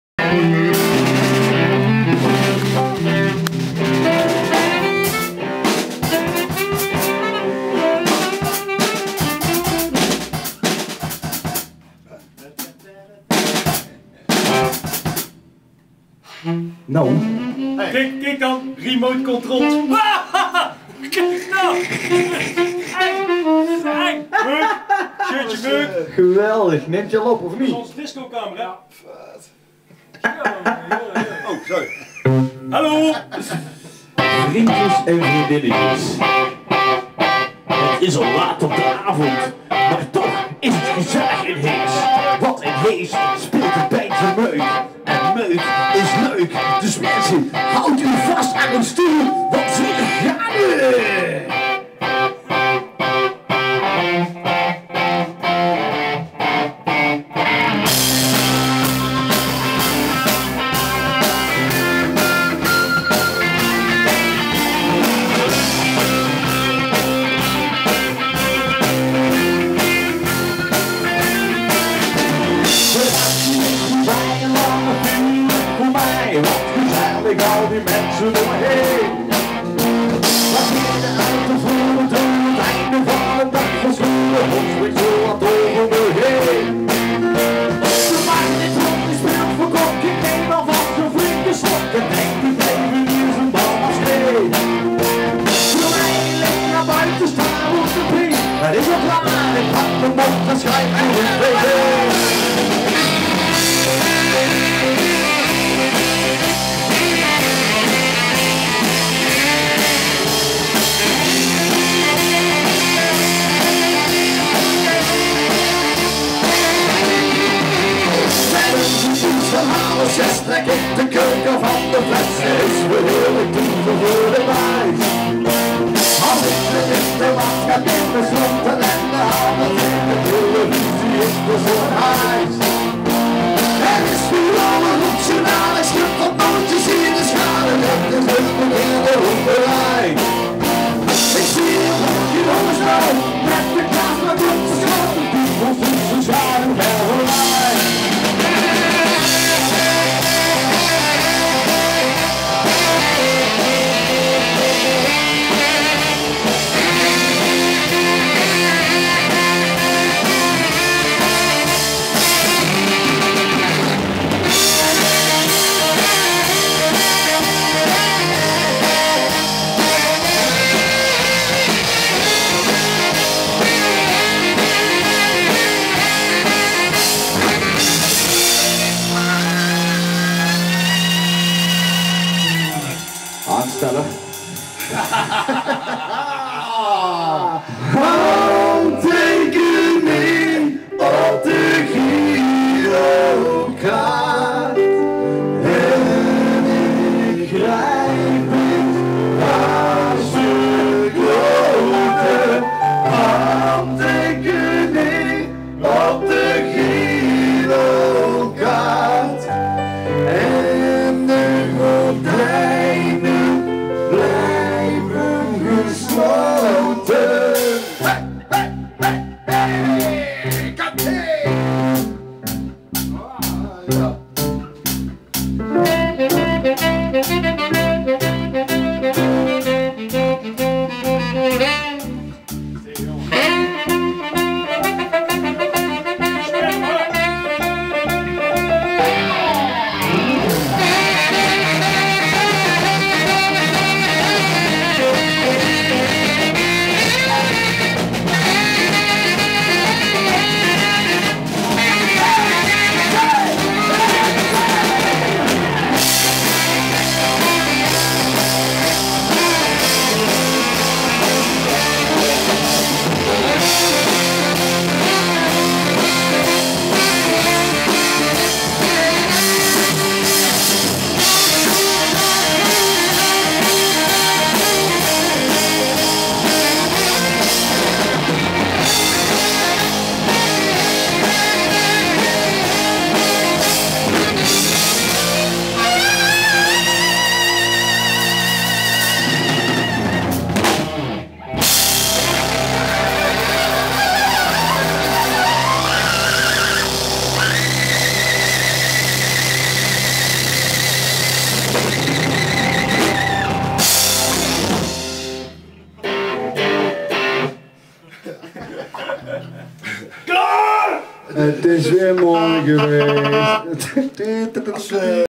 UNIEKE BEELDEN: repetitie MEUK met Roberto Totally Oversaxed
UNIEKE BEELDEN! MEUK repeteert voor een speciaal gastoptreden met Robert van Asseldonk, alias "Roberto Totally Oversaxed" bekend van de BZB!

Hierbij de rauwe, unieke en ongemonteerde beelden uit het oefenhok, met de draaiende webcam!!